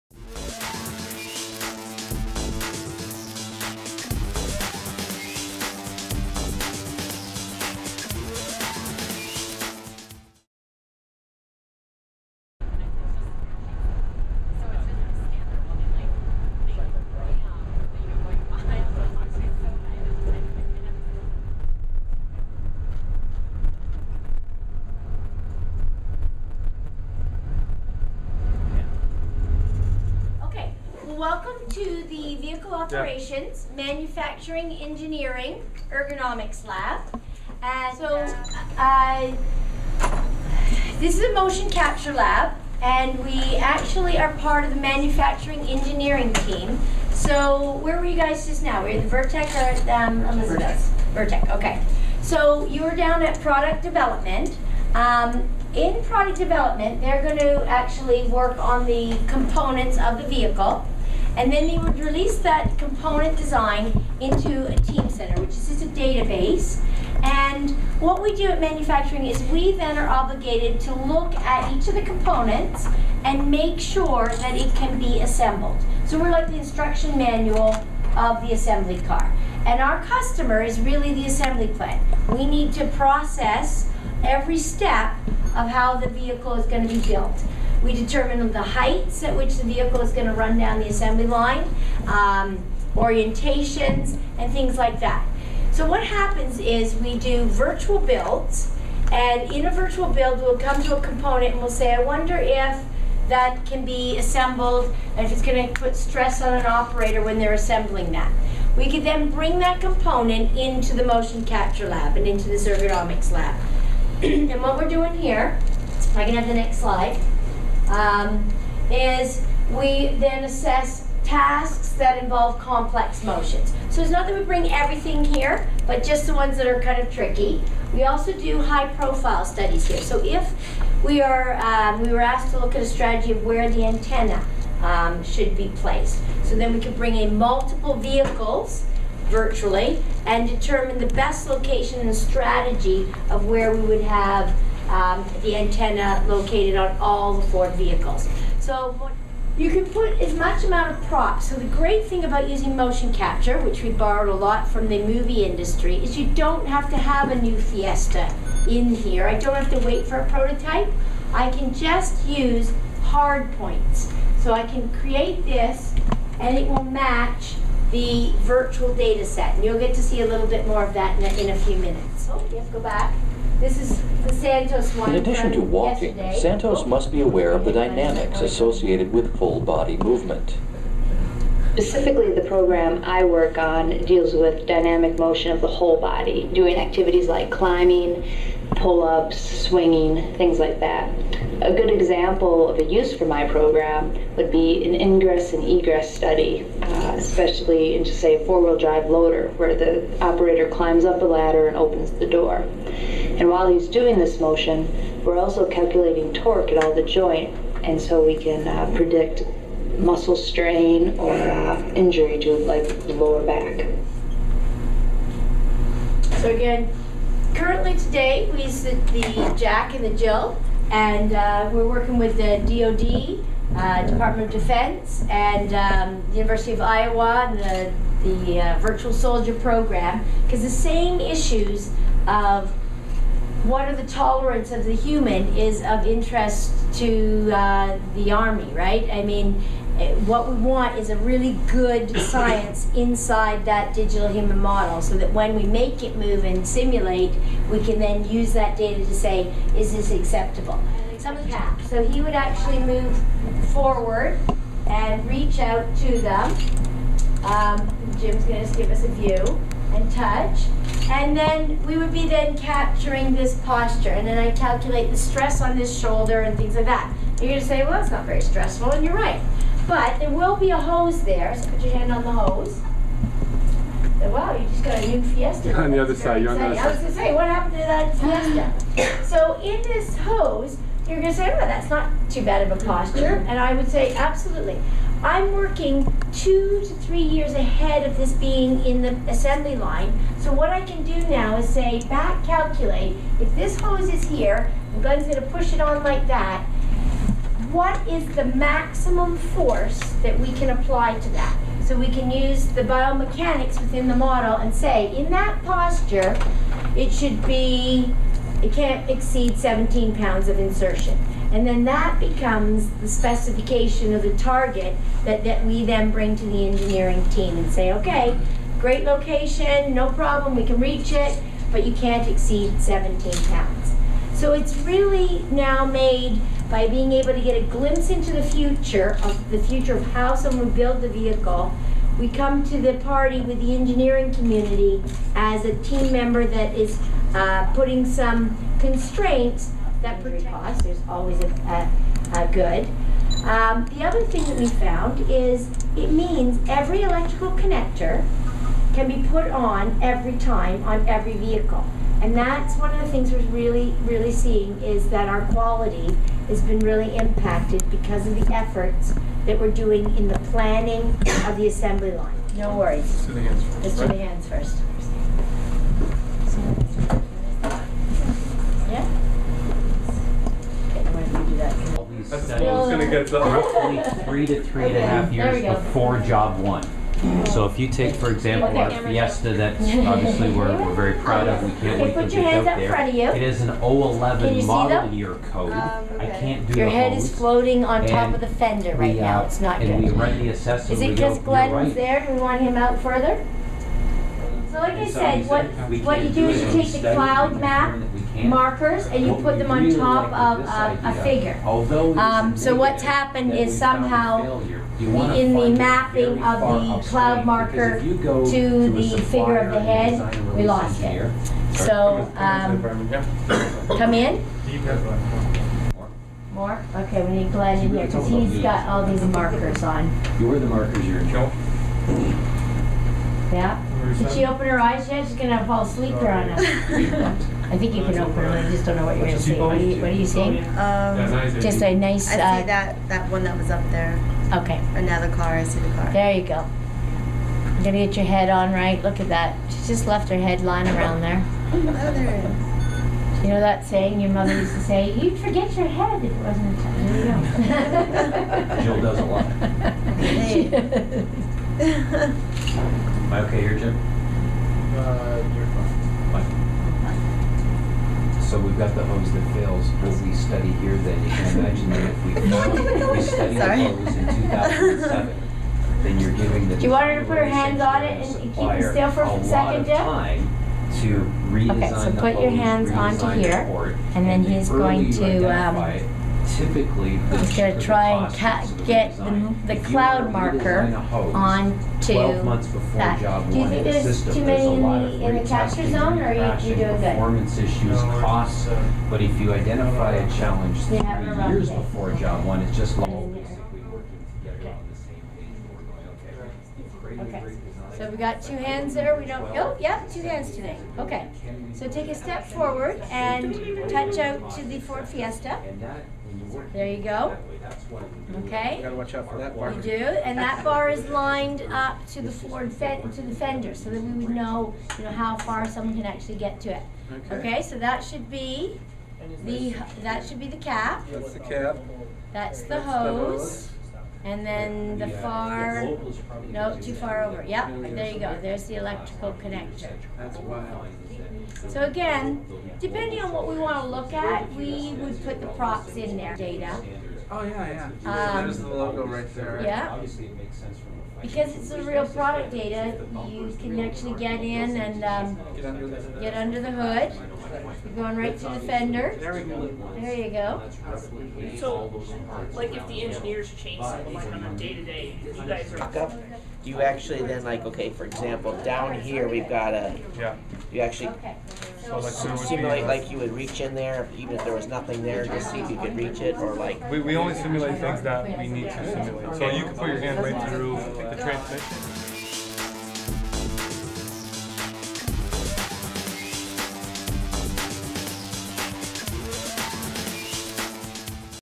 Ford Virtual Manufacturing
We look at the Motion Capture room and how you can Virtually play around a Ford Fiesta still on the drawing block